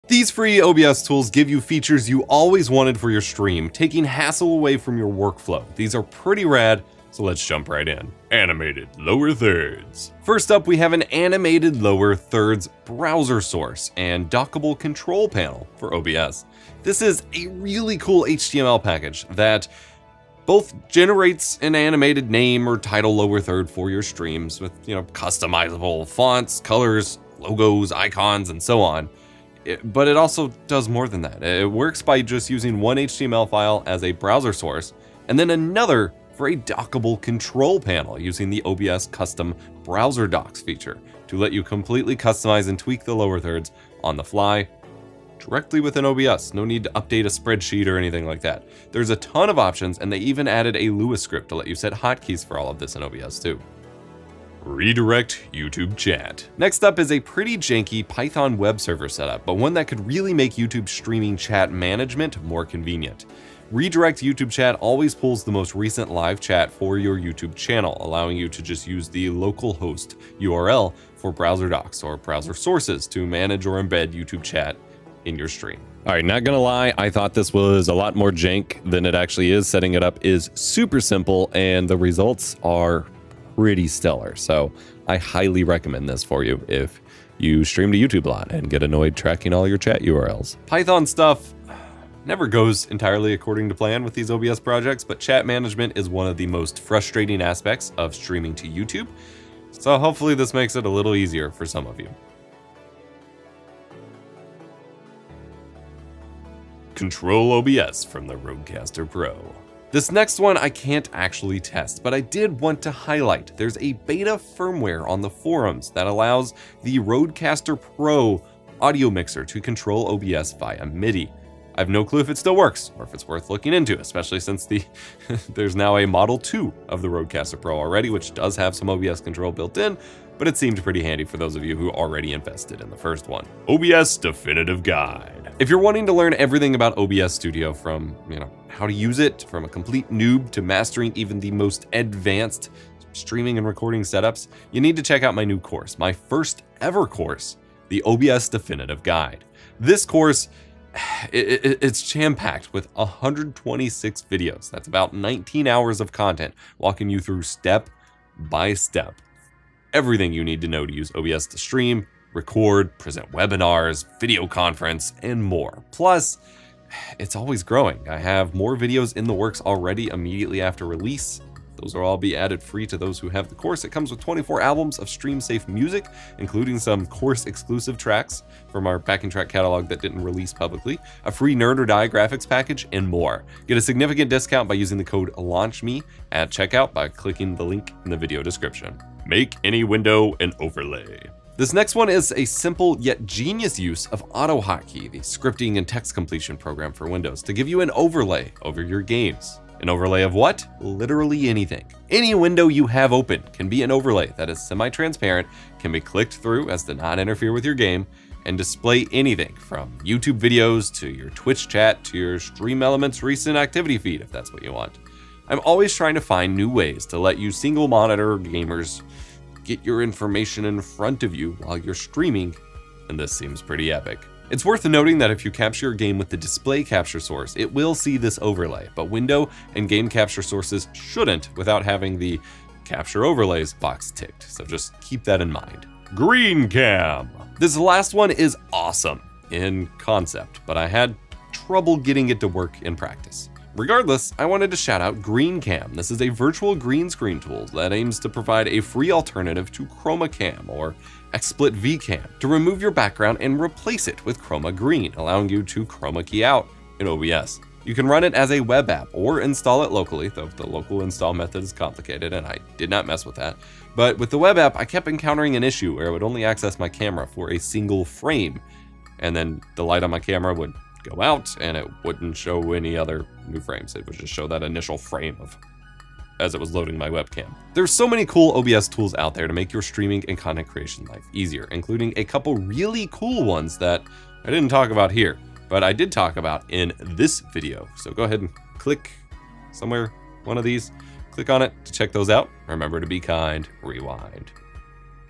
5 Free OBS tools your stream setup is MISSING
Free OBS Tools is BACK! Here's some free tools to give even more workflow enhancement to OBS Studio.

TIMECODES:
00:12 Animated Lower-Thirds
00:54 Redirect YouTube Chat
01:48 Control OBS from Rodecaster Pro
02:14 OBS Definitive Guide
03:09 Make any window an overlay
03:56 Remove background without Green Screen